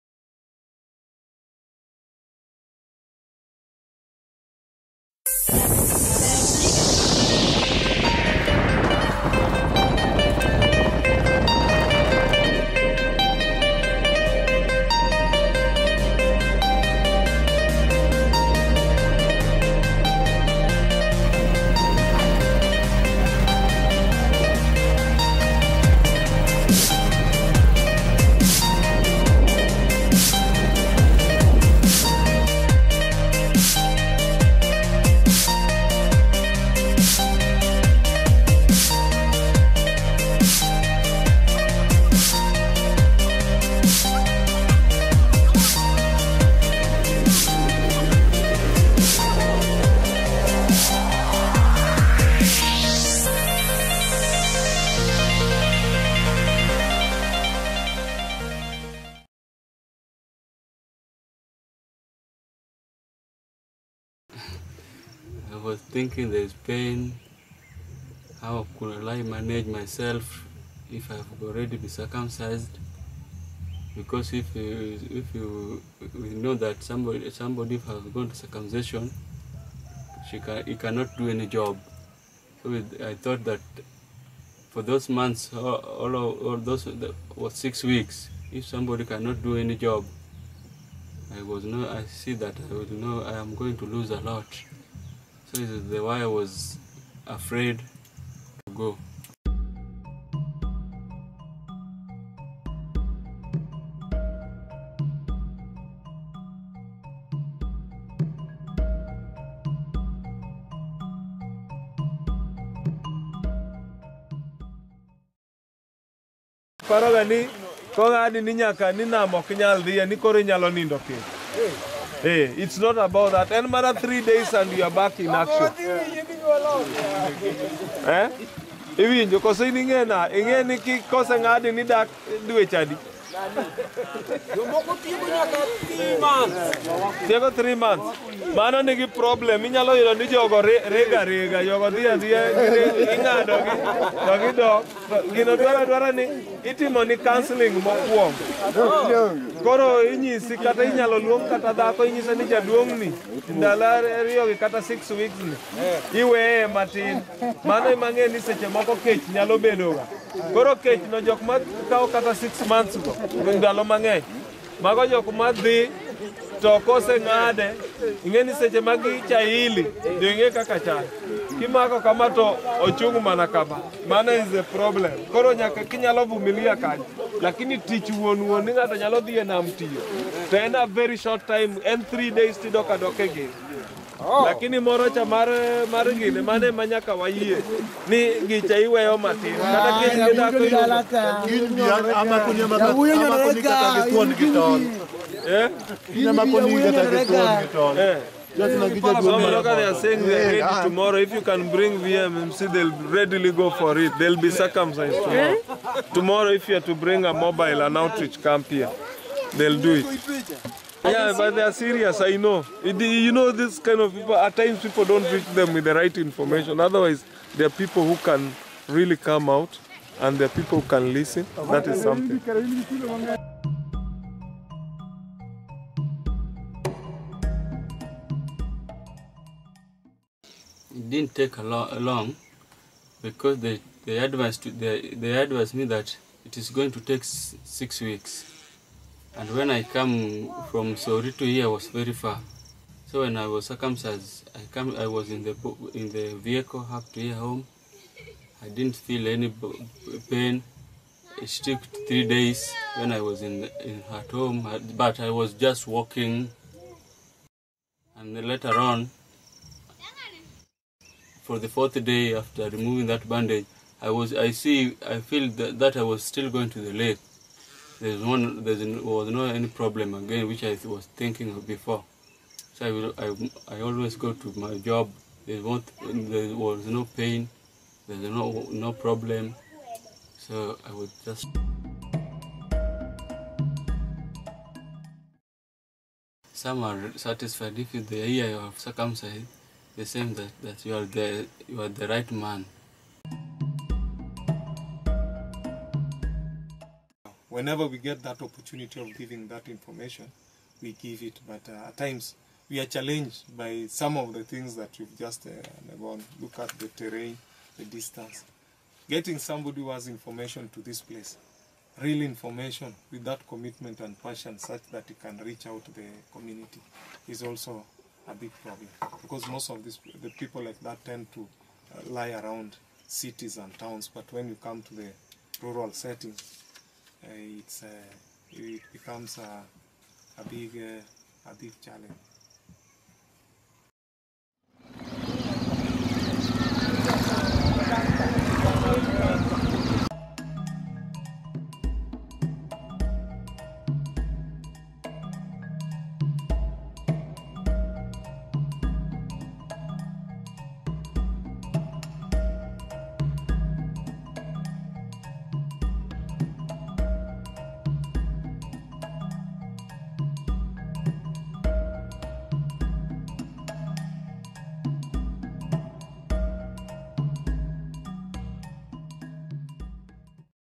Male Circumcision
Pamoja Together traveled to the remote fishing villages on the shores of Lake Victoria. Fishing communities in low/middle income countries constitute one of the highest risks groups for HIV/AIDS. Evidence suggests male circumcision reduces risk of heterosexually acquired HIV infection in men by 60%. Yet, few HIV/AIDS programs have been targeted in these communities.

WHO/UNAIDS recommends male circumcision as an effective intervention for HIV prevention. But many myths about negative effects of circumcision make these fishermen afraid to be circumcised.

"I was thinking there is pain. How could I manage myself if I have already been circumcised? Because somebody who have a circumcision, he cannot do any job. I thought that for those 6 weeks, if somebody cannot do any job, I see that I am going to lose a lot. So this is why I was afraid to go." -- Collins, a local

Collins also revealed that some men even believe they will be unable to have a child if circumcised.

Erick Okoima is Kenya's National Coordinator for the International AIDS Candlelight Memorial. He is a person living with HIV and a strong advocate for the prevention of HIV transmission among the communities in Western Kenya.

Erick Okioma: Now, you think that you can't go back to the lake if you are circumcised? It is not about that, it is only for 3 days and you are back in action. Listen. When you are circumcised, how many months do you think you stay in that state?

Fisherman: There are some people who stay up to 3 months.

Erick Okoima: Three months? Those people have a problem. You can find out that those people are just running around. They are circumcised, but then go back to work immediately. This thing needs you to get counseling first. Now you are told during counseling, or even a woman might be told. During this 2 days for this 6 weeks, leave this man along a little bit. This is when I know hunger for sex might set in. Now this hunger for those that take 6 weeks or a long time, these are people that when they have been circumcised and that thing (penis) is itching them. You know how it happens, that when you touch it, it stands straight pointing ahead! That is the problem. If you can persevere at this point it should be alright. About 3 days and you are back to work. But the other one (sex) is what we must accept, that we have to abstain a little bit.

Fishermen: Just come, even tomorrow and will catch them even by force with ropes. Tomorrow I will catch them for you by force with a rope.

Erick Okoima: They are saying tomorrow if you can being the mobile circumcision clinic they will readily go for it. They'll be circumcised tomorrow. Yeah they are serious, I know. You know at times people don't reach them with the right information. Otherwise they are people who can really come out and listen.

Erick Okoima managed to convince Collins to get circumcised by providing the correct information and guidance through an outreach camp.

Collins: It didn't take long because they advised me that it is going to take 6 weeks. And when I come from Sori to here (Gwassi) was very far. So when I was circumcised I was in the vehicle up to here home. I didn't feel any pain. I slept for three days when I was at home, but I was just walking. And then later on, for the fourth day after removing that bandage, I feel that I am still going to the lake. There was no problem like I was thinking before. So I always go to my job and there was no pain. There was no problem. So I was just satisfied.

--What reaction do you get from women after being circumcised?

Collins: Some are satisfied. If they hear you are circumcised, they say you are the right man.

--What are some of the challenges in dispelling these myths about circumcision?

Erick Okoima: Whenever we get that opportunity of giving that information we give it. But at times we are challenged by some of the things that you have just gone through. Look at the terrain, the distance. Getting someone who had information to this place, real information with that commitment and passion such that he can reach out to the community is also a big problem. Because most of the people like that tend to lie around cities and towns, but when you come to the rural setting then it becomes a big challenge.

The United States President's Emergency Plan for AIDS Relief (PEPFAR) provides the funds for male circumcision programs in Western Kenya.

Access to these villages is not easy, but getting the correct information and medical services to these fishing communities is key in the prevention of HIV transmission.